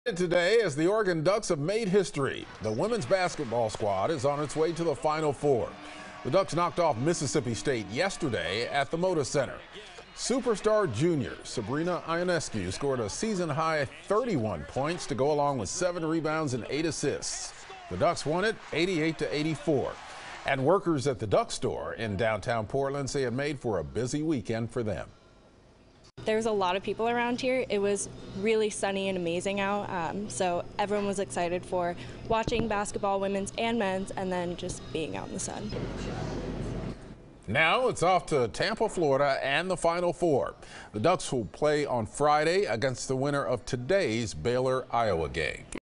Ducks dance in Final 4, fans flock to get gear
With the Oregon Ducks in their first NCAA Women's Tournament Final 4, fans need new gear to be ready for the weekend